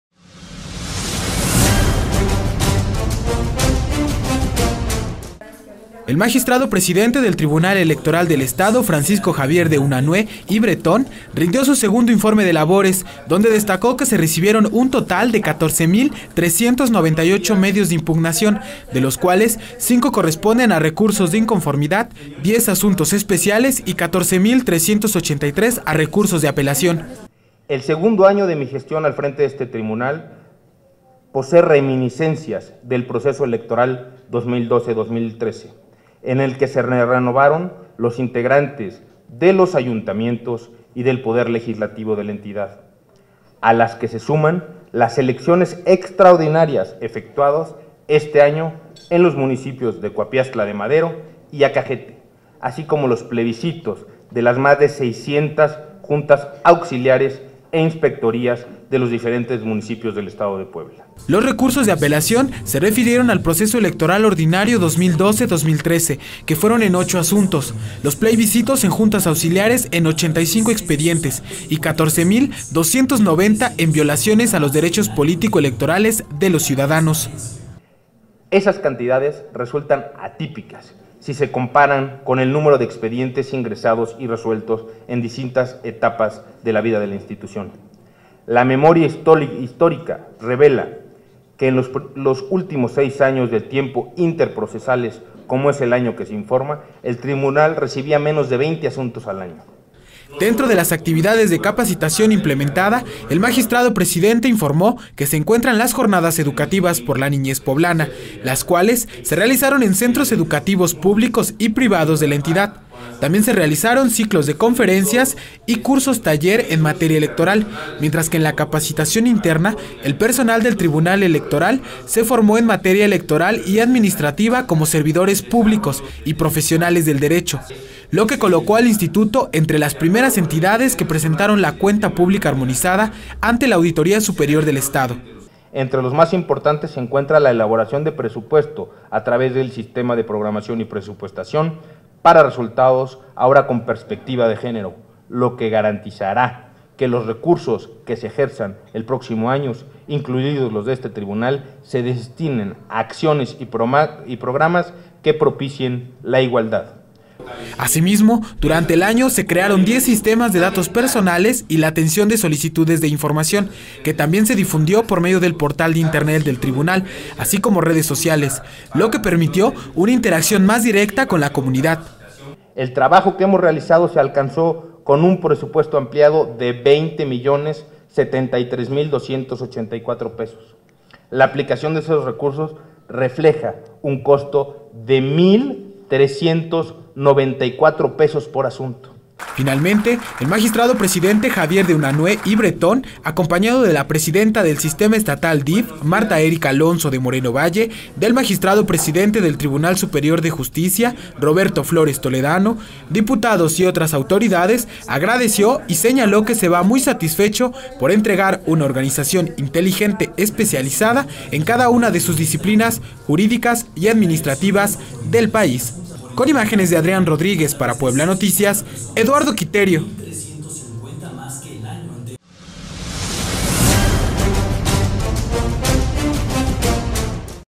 Presenta su segundo informe de labores Francisco Javier de Unanue y Bretón
Recursos de Inconformidad y 10 Asuntos Especiales y 14 mil 383 a Recursos de Apelación